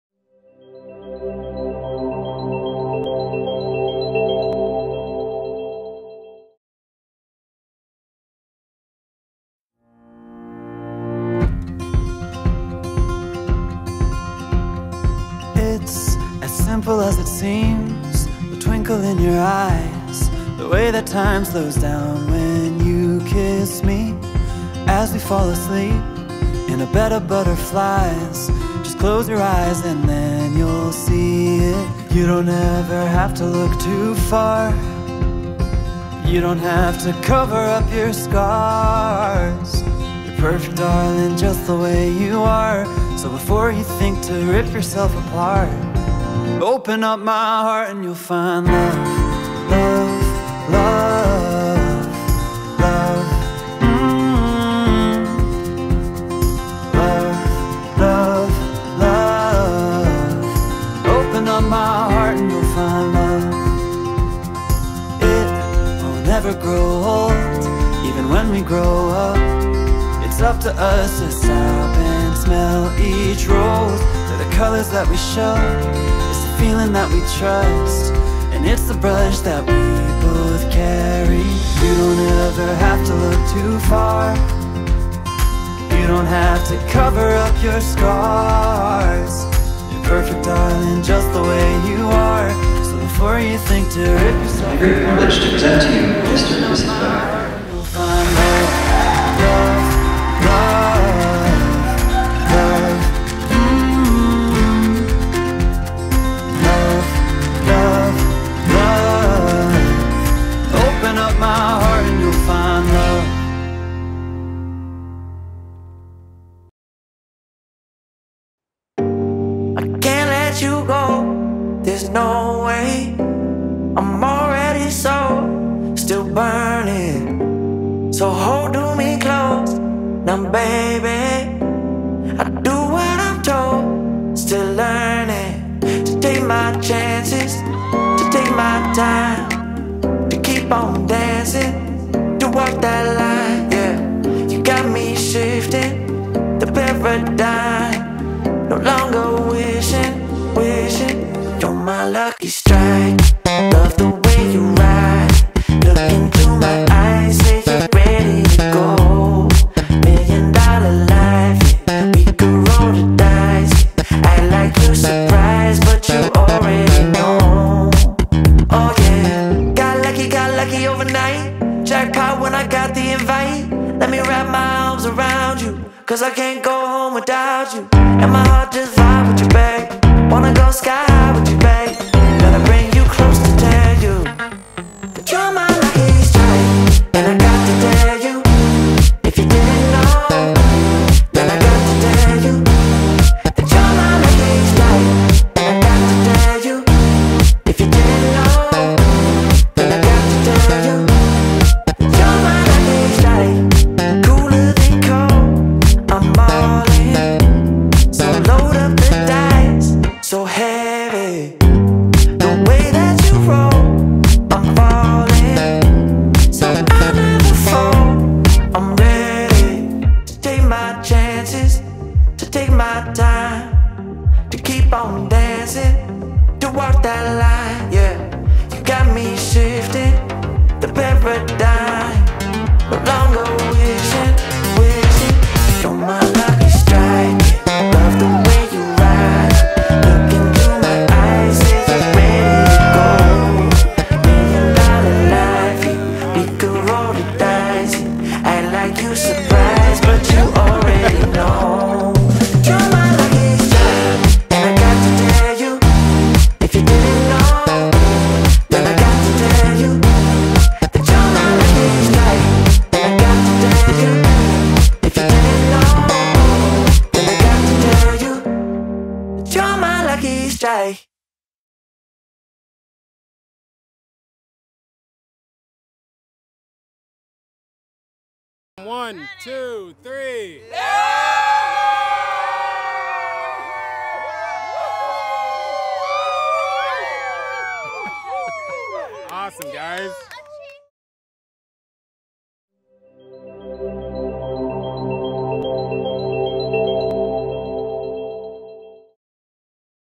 07 17 2021 Michael & Jessica Highlight
Michael & Jessica had a beautiful wedding reception at Cherry Creek Golf Course in Shelby Township Michigan.  JD Entertainment was there to film the day!  We hope you enjoy this wedding video as much as enjoyed capturing the wedding events!